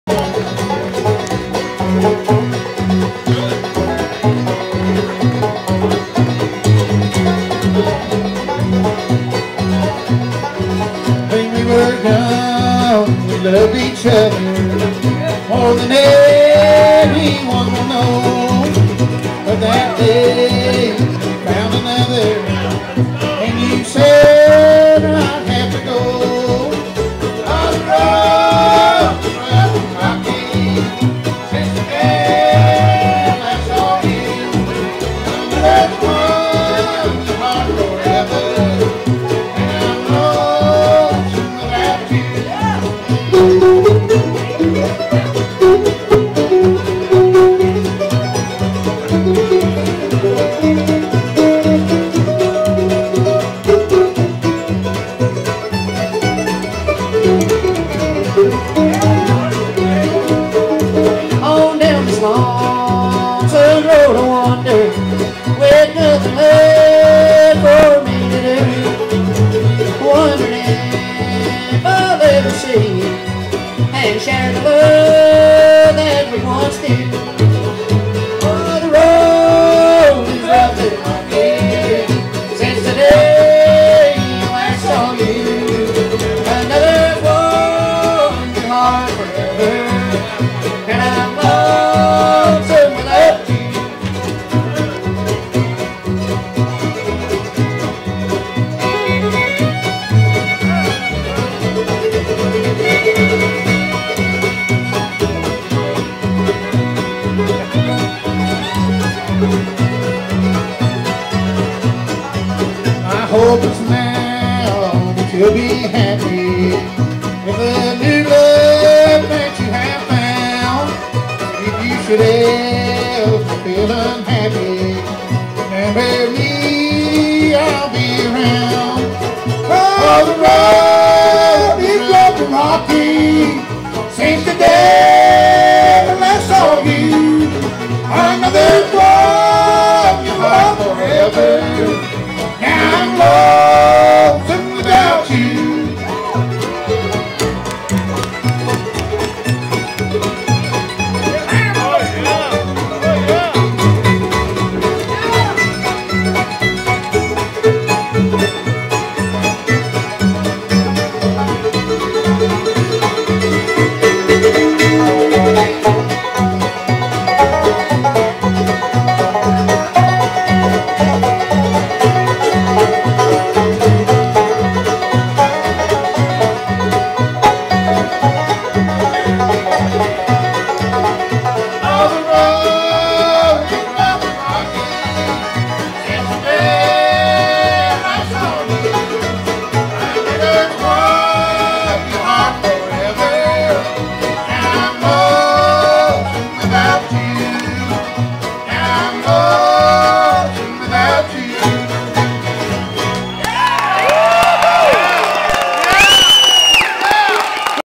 Movie Bands
Bluegrass Brothers, Larry Gillis, Junior Sisk rock the Plymouth Stage